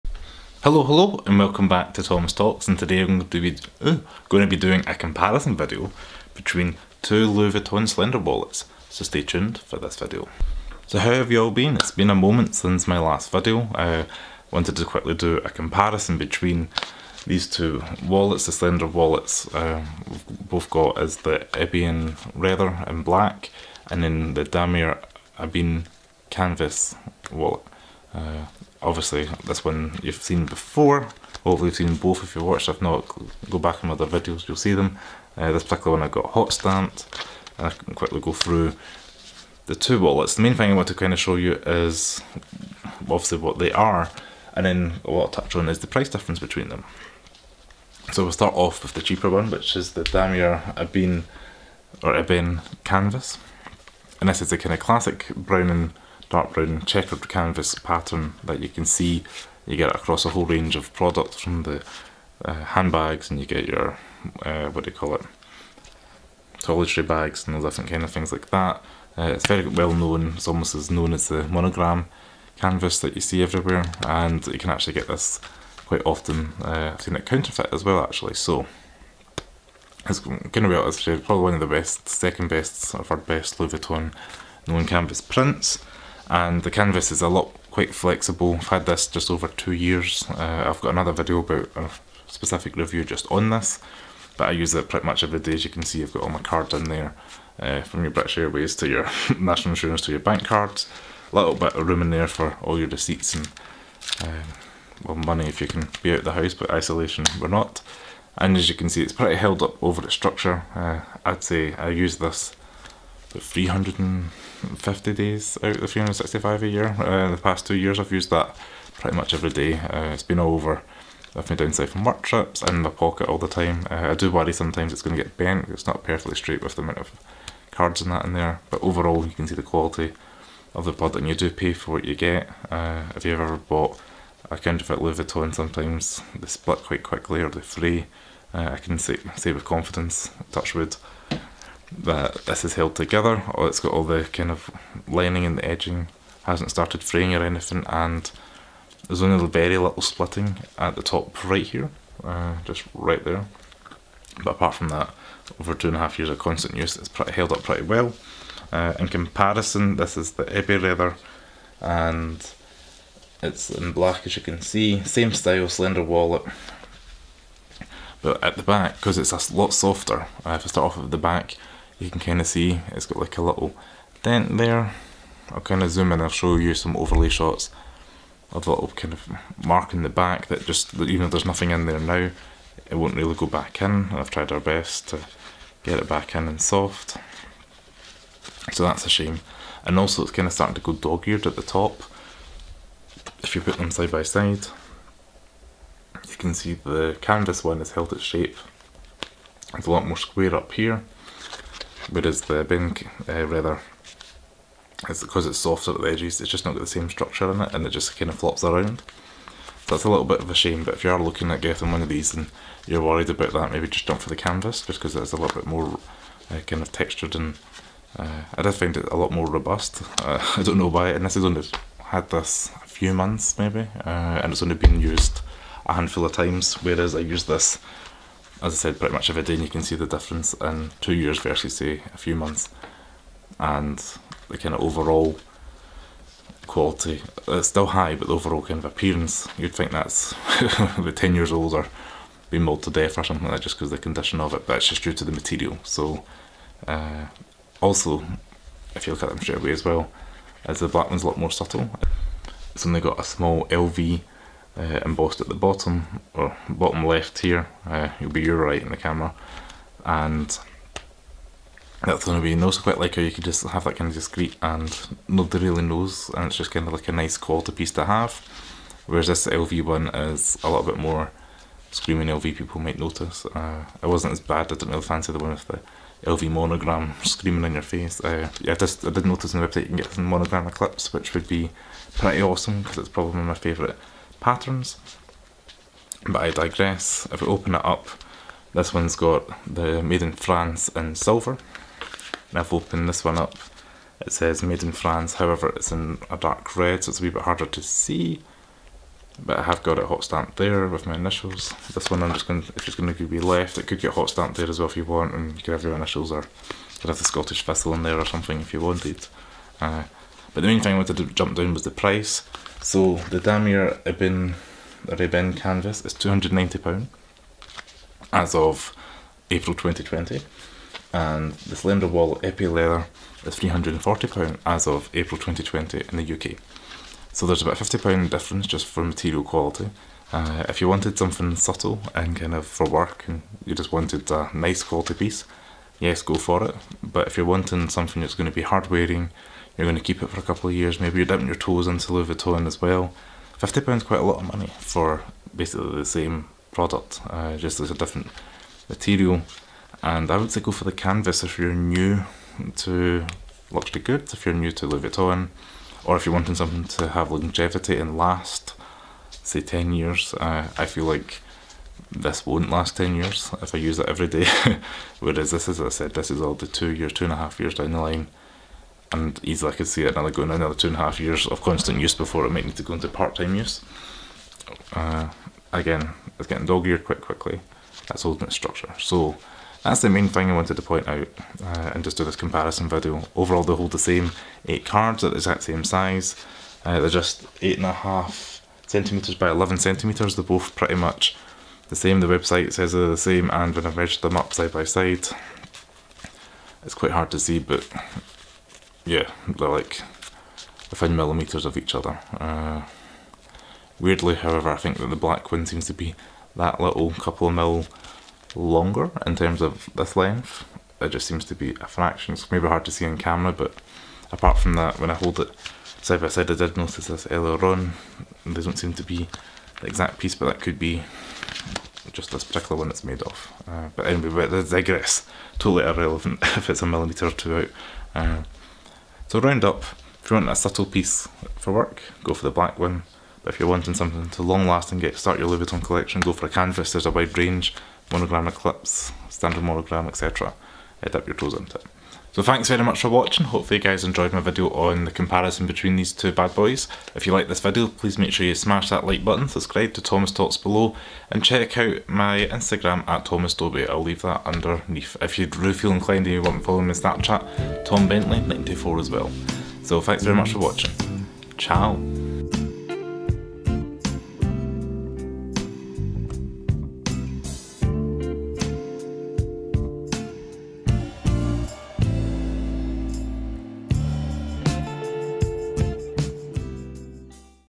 Louis Vuitton Leather vs Canvas wallet comparison
Hello Hello,

In this video I compare the EPI leather slender LV wallet against the LV Canvas slender wallet. The leather one is currently sitting at £340 and the canvas £290 but is it worth the extra?

I hope you all enjoyed this comparison video between the two pieces?